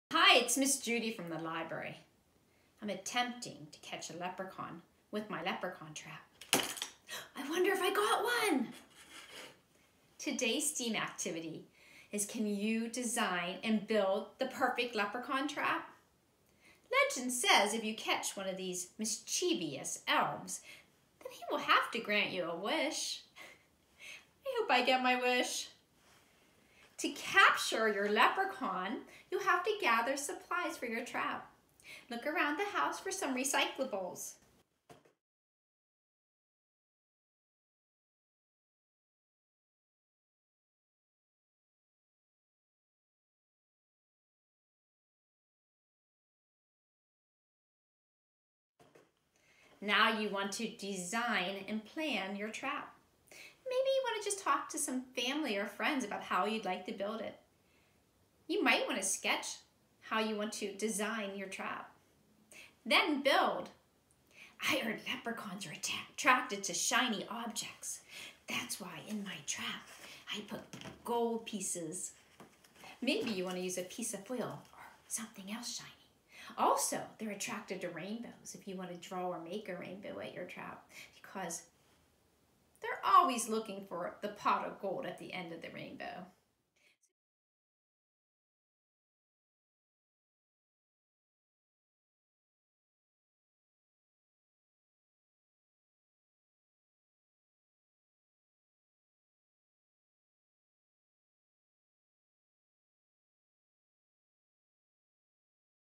STEAM Challenge | Leprechaun Traps
Use your imagination and engineering skills to catch a leprechaun.  Construct the perfect trap to catch these mischievous elves!